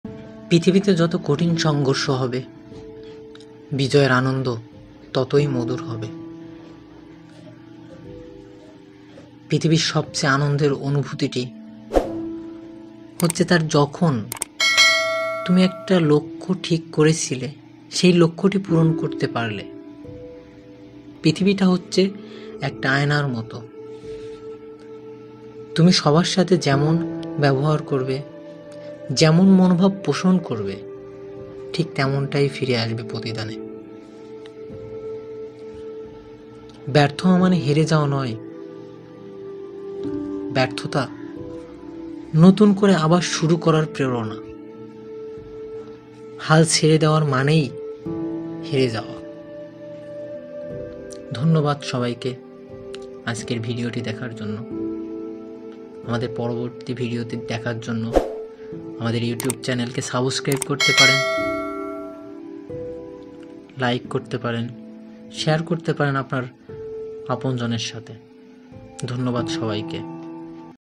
ব্যর্থ হওয়া মানে হেরে যাওয়া নয় ।। Best Motivational Speech in Bangla ।। SKS Goldy
যত কঠিন সংঘর্ষ হবে,
 বিজয়ের আনন্দ ততই মধুর হবে

ব্যর্থ হওয়া মানে হেরে যাওয়া নয়, ব্যর্থতা
 নতুন করে আবার শুরু করার প্রেরণা।
 হাল ছেড়ে দেওয়া মানেই হেরে যাওয়া।

Thanks